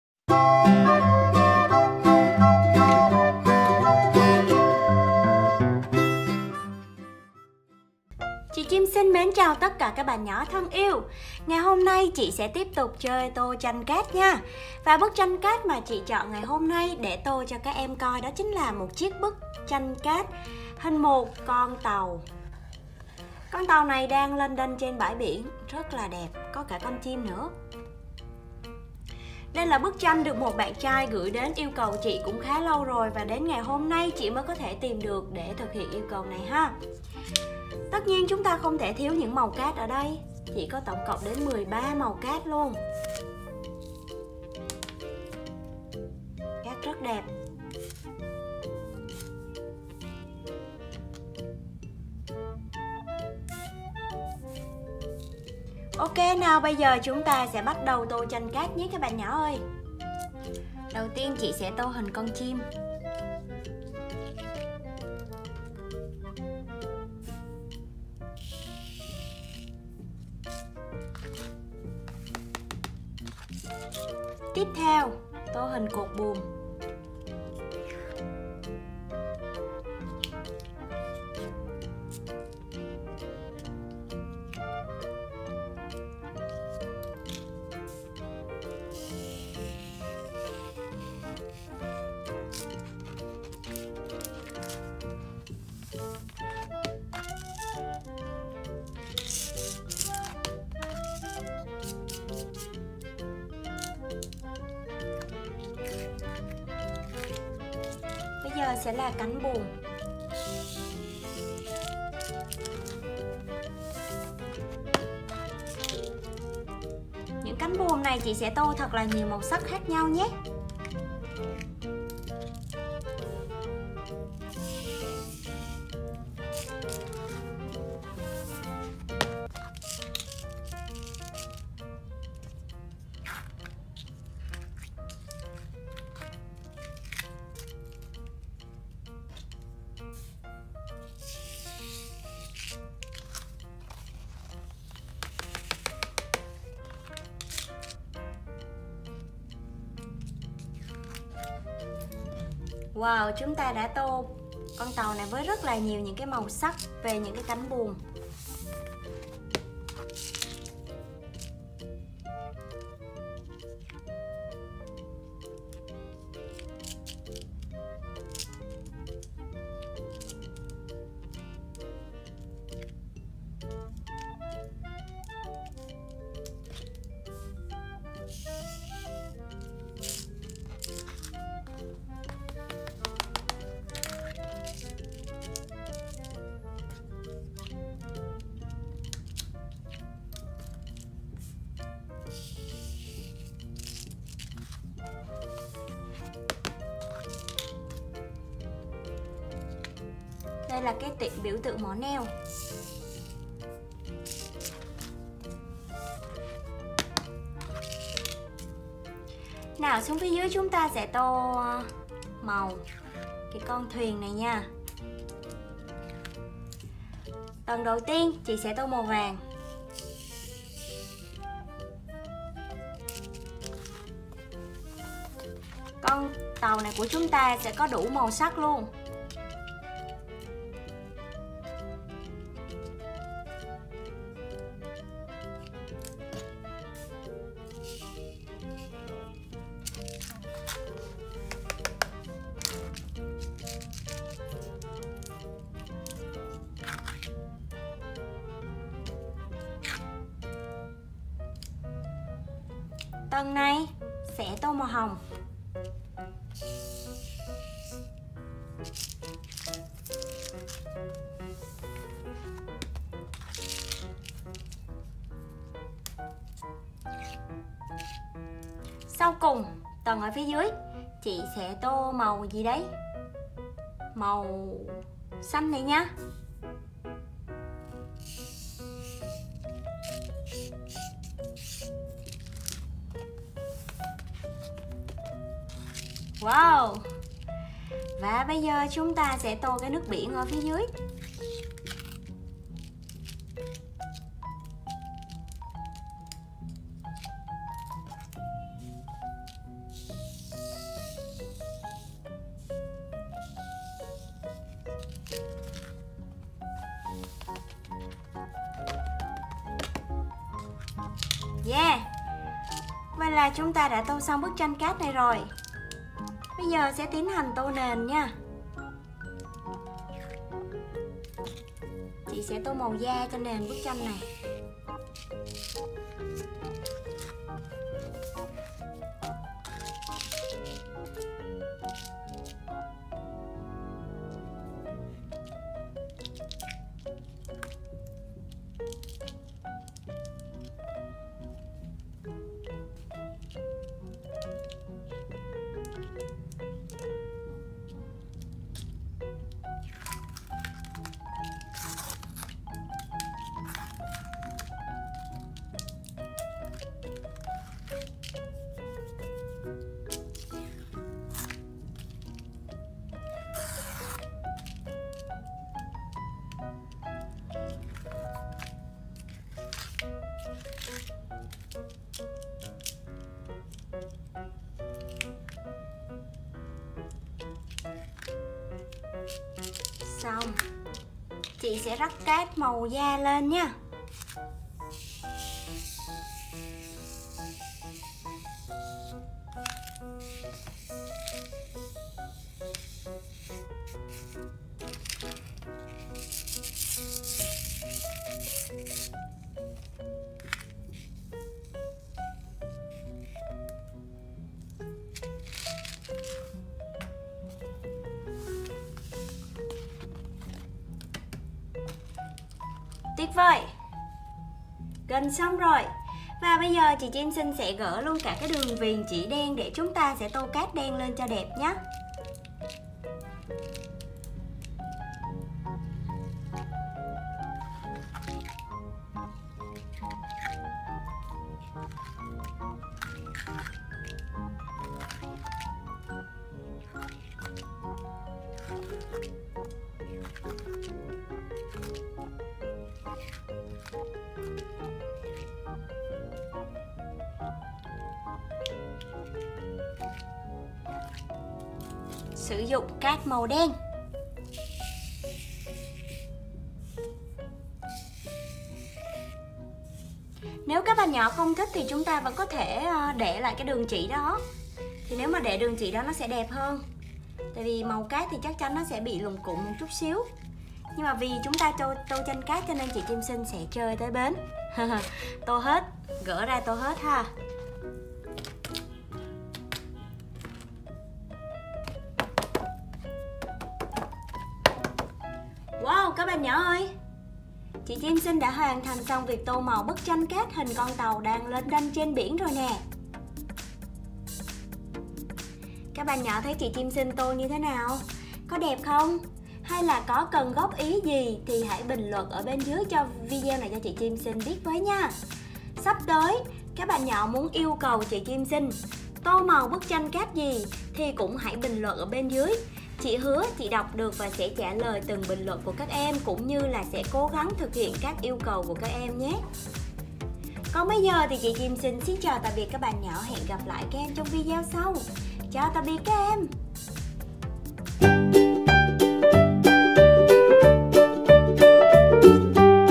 Đồ chơi trẻ em TÔ MÀU TRANH CÁT CON TÀU CÁNH BUỒM - Sand painting ship toys kids (Chim Xinh)
Chị Chim Xinh tô màu tranh cát con tàu cánh buồm mạnh mẽ, có rất nhiều màu cát được sử dụng trong bức tranh này. Các em xem video và cho chị ý kiến nhé.